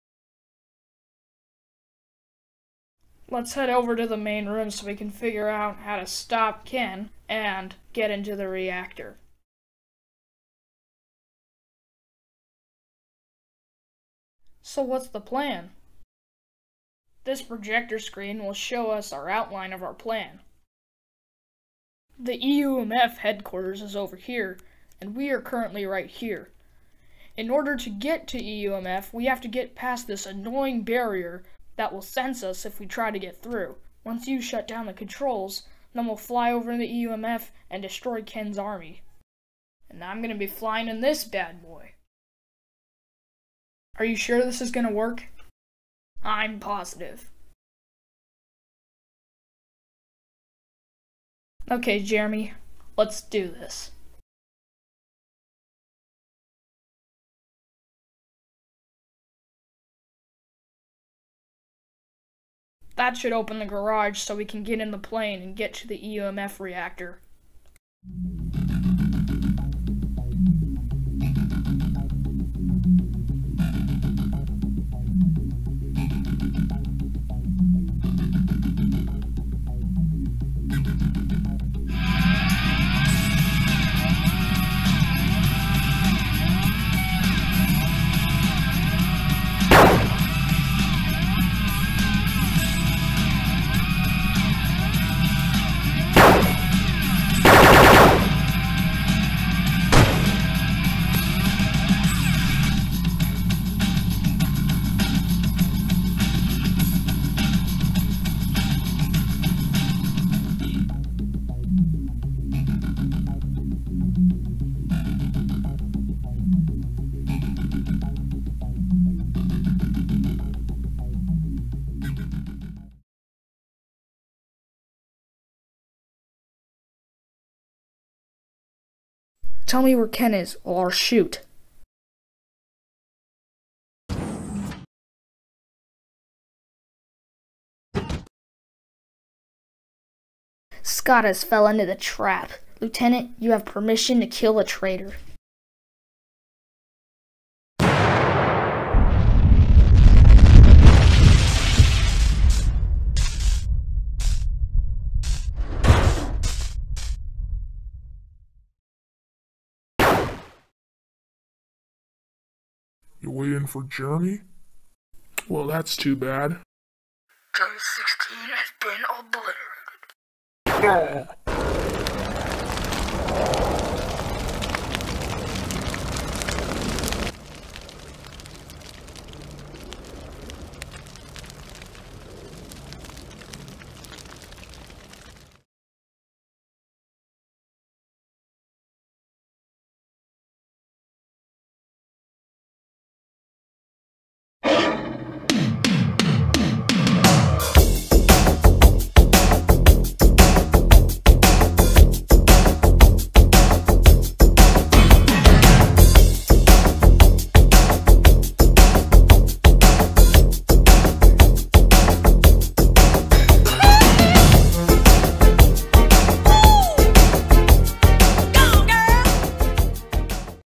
The Opposition 5 (Stop Motion Story)
This is a Re-upload because I messed up at the very end during the credits, so here is the video. *Sorry if the voice acting is a little bad. I am now making my Star Wars series, so Episode 1 should come out soon!